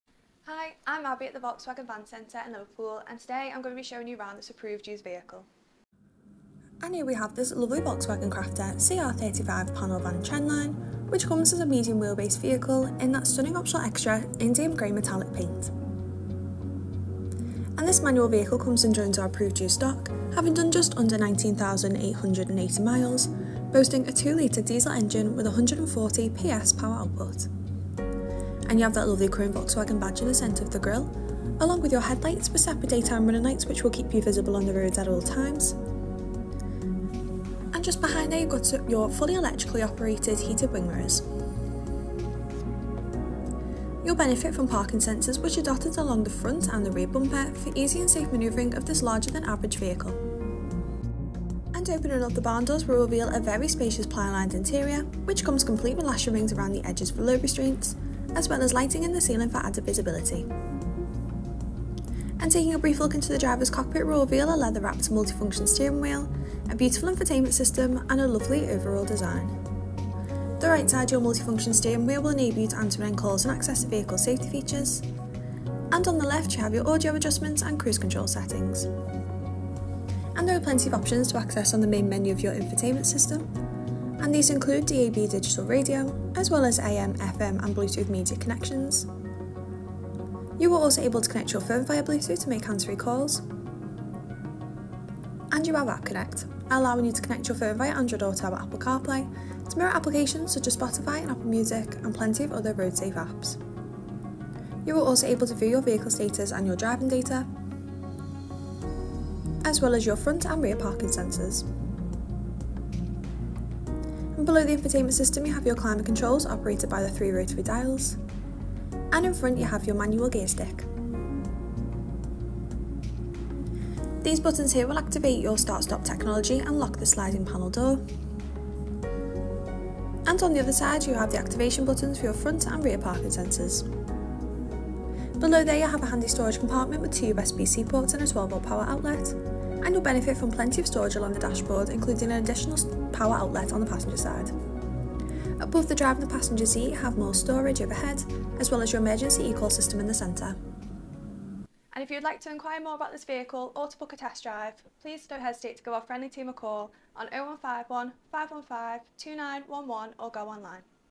Volkswagen Crafter Cr35 Mwb Diesel Fwd 2.0 TDI 140PS Trendline | Volkswagen Van Centre Liverpool
Check out this Volkswagen​​ Crafter Trendline, currently for sale at Volkswagen

Fuel: Diesel
Transmission: Manual
Paint: Indium grey
Registration: DG72NKH

Features:
- Multi-functional Steering Wheel
- Front & Rear Parking Sensors
- Front Assist
- City-Emergency Braking System